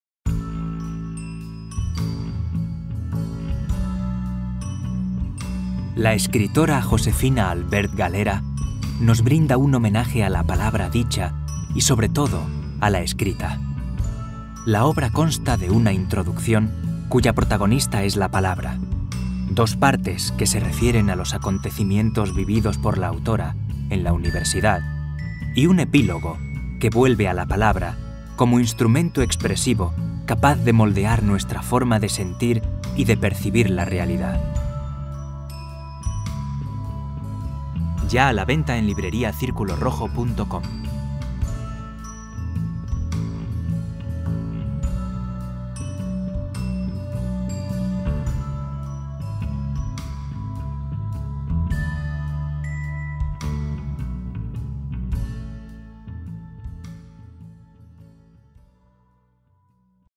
Flor de azar (Booktrailer) - Editorial Círculo Rojo
El libro quiere ser un homenaje a la palabra dicha y, sobre todo, a la palabra escrita. Diez años de silencio ha tardado la palabra en hacerse signo visible para contar una historia que sin ella no existiría. «He intentado --escribe la autora-- decir mi razón con palabras claras, llanas y significantes y establecer una alianza entre historia y literatura, sucesos y palabras». En ocasiones, la historia fluye dentro de la literatura y en otras el episodio apunta al hecho literario cuando la vida real se hace escritura.
La obra consta de una Introducción, cuya protagonista es la palabra, dos partes, que se refieren a los acontecimientos vividos por la autora en la Universidad Rovira i Virgili de
Tarragona, durante el periodo que va desde las pruebas de acceso a la Universidad en 1998 hasta, aproximadamente, 2002, y un Epílogo, que vuelve a la palabra, como instrumento expresivo capaz de moldear nuestra forma de sentir y de percibir la realidad; la palabra también como un poderosísimo instrumento de poder y al
mismo tiempo como creadora de mun dos de libertad y de ensueño. Los episodios no siguen un orden cronológico, sino que van apareciendo al alimón de las circunstancias que el recuerdo va colocando en primer plano. Los hechos y los personajes que desfilan por esta historia, cuando, por comparación o por contraste, guardan una cierta relación o semejanza con episodios históricos y/o literarios, se recoge la cita y el comentario pertinente (Cervantes, Unamuno, Calderón de la Barca, Dámaso Alonso, Quevedo; la Odisea y la Eneida, Horacio y Garcilaso; Sebastián de Covarrubias y Baltasar Gracián, etc.). Varias viñetas de Faro y Napi, con su iterpretación en clave de humor de algunos episodios, dan a la obra un tono suave y distendido, que finaliza con los Anexos, que dan testimonio del contenido del libro
El Prólogo ha sido escrito por D. Francisco Caja, Profesor de la Universidad de Barcelona.